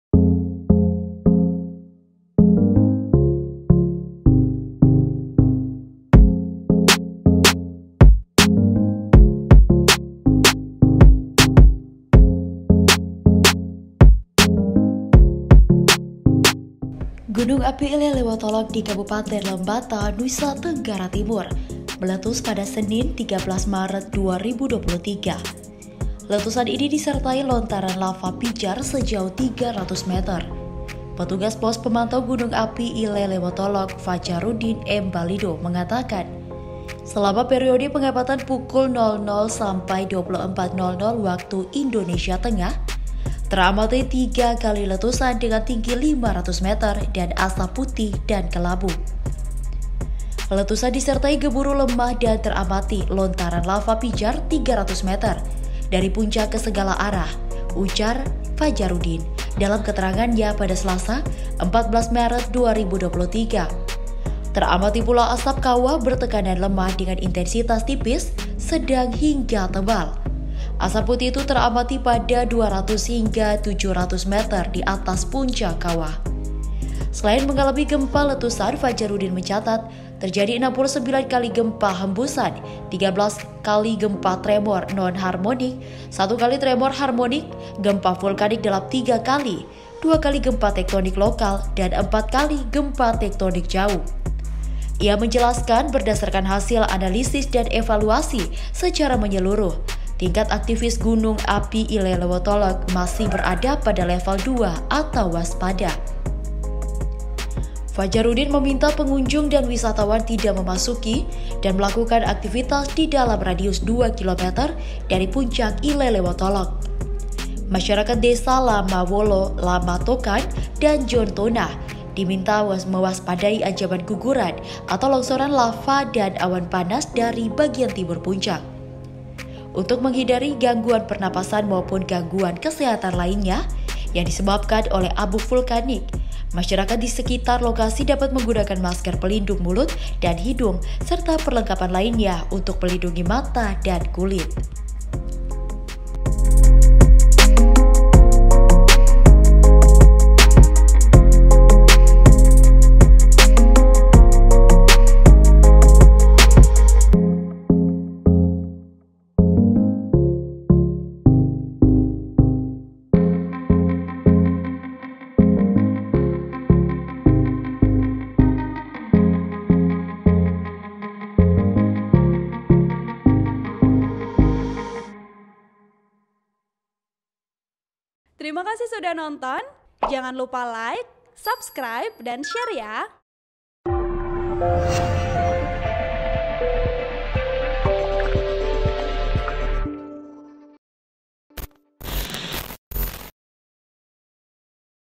Gunung Ile Lewotolok Lembata Mengamuk Lontarkan Lava Pijar Senin Malam
TRIBUNFLORES.COM, LEWOLEBA-Gunung Api Ile Lewotolok di Kabupaten Lembata, Provinsi Nusa Tenggara Timur (NTT) 'mengamuk' melontarkan lava pijar pada Senin 13 Maret 2023 pukul 24.00 Wita.

Pos Pengamatan Gunung Api (PPGA) Ile Lewotolok di Desa Laranwutun, Kecamatan Ile Ape melaporkan letusan tersebut terjadi sebanyak tiga kali dengan tinggi kolom abu 500 meter. Lontaran lava pijar akibat letusan itu diperkirakan sejauh 300 meter di atas puncak ke segala arah.

Artikel ini telah tayang di Tribunflores.com dengan judul Gunung Ile Lewotolok Lembata Mengamuk Lontarkan Lava Pijar Senin Malam,

Penulis: Ricko Wawo
Editor: Egy Moa
VP: Tian Kedoh